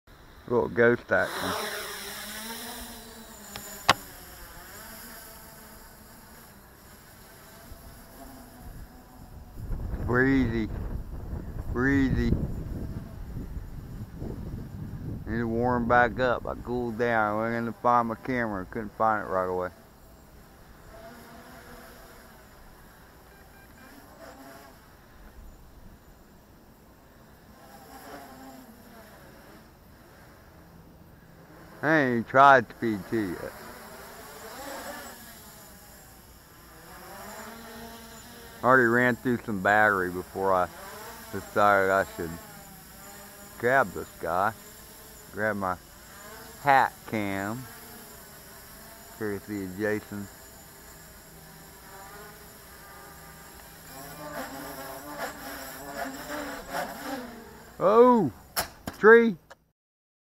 20180301 2015 F100 1stbattery Cut1
F100Ghost 2nd Flight, First battery.  I had already put her up then decided I should document via hat cam.  I did not crash on the first flight but landed quite well which is more than I can say for this clip.